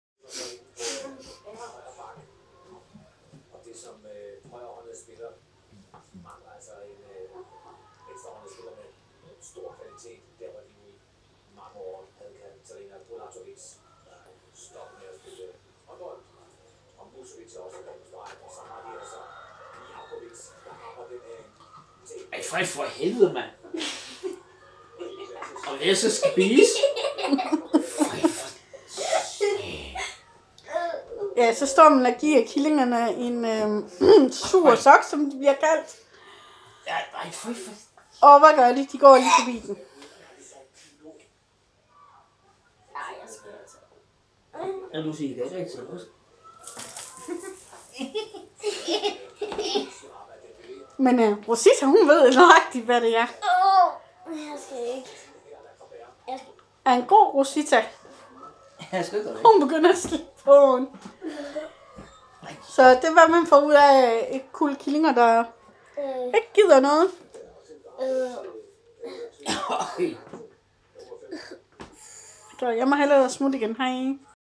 Killinger 8 uger i morgen!
Ella og Ena bliver 8 uger gl i morgen! Vildt som tiden går og så frække de 2 piger er.